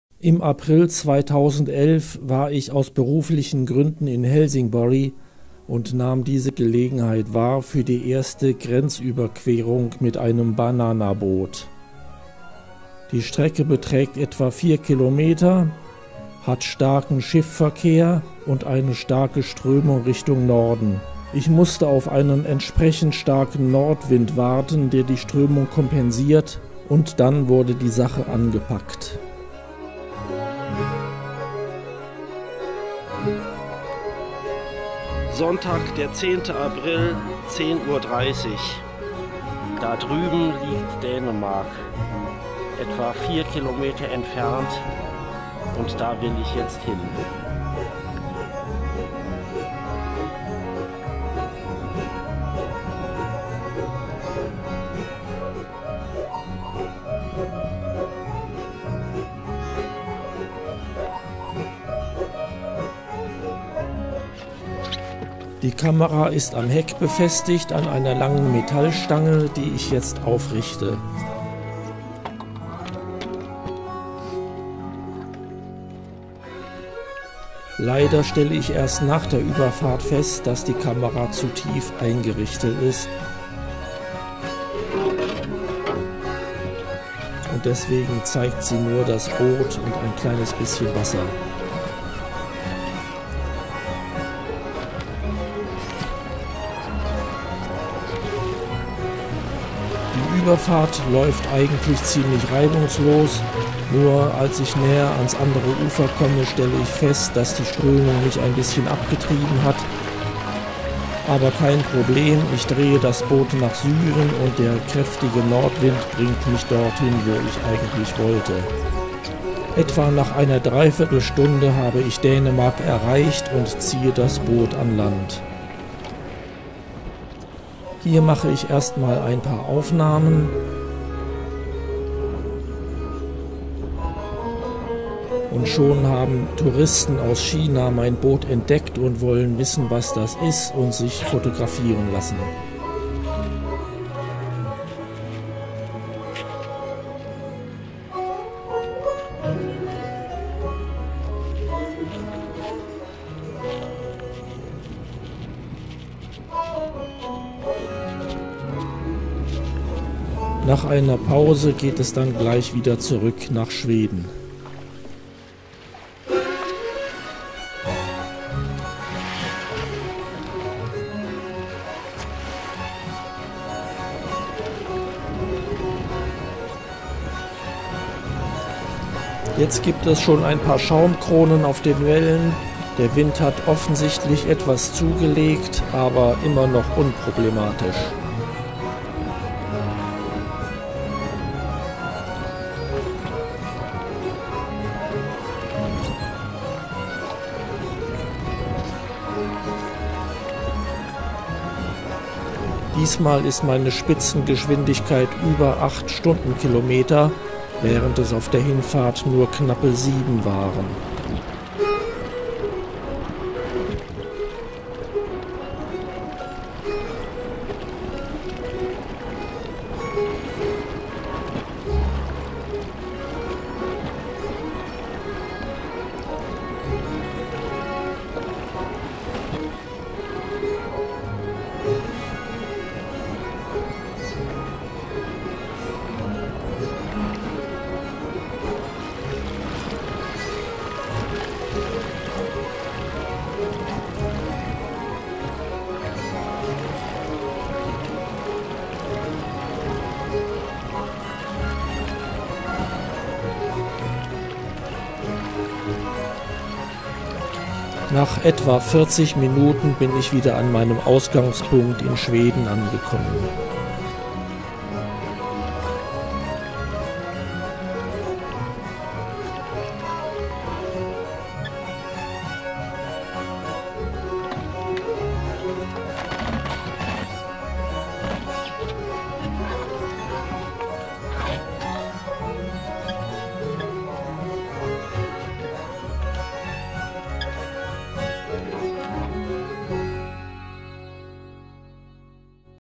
Öresund-Querung mit dem Banana Boot
Die wohl erste Staatsgrenzenüberquerung mit einem Banana-Boot!